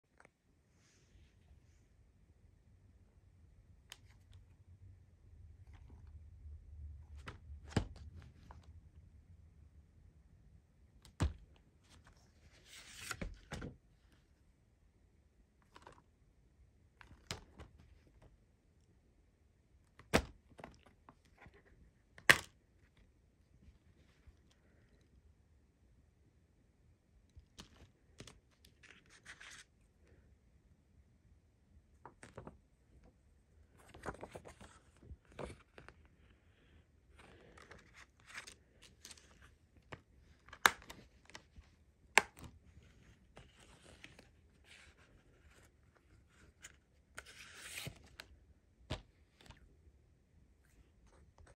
Blu-Ray Update for July 2nd, 2024
I got 1 Blu-Ray from Wal-Mart today!